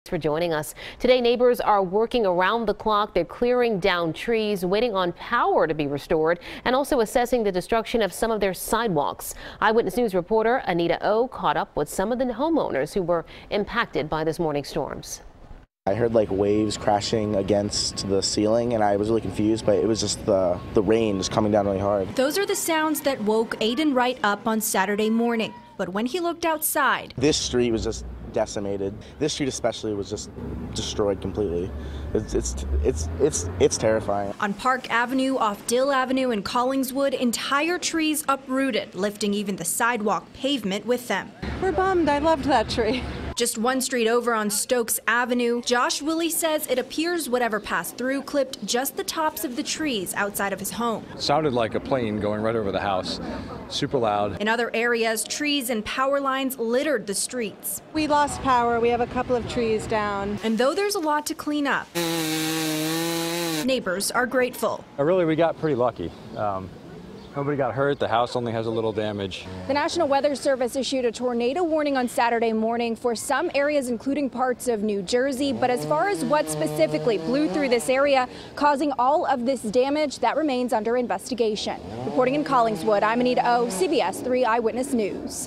Powerful Storms Bring Down Trees, Cause Flooding, Power Outages
CBS 3's Anita Oh reports.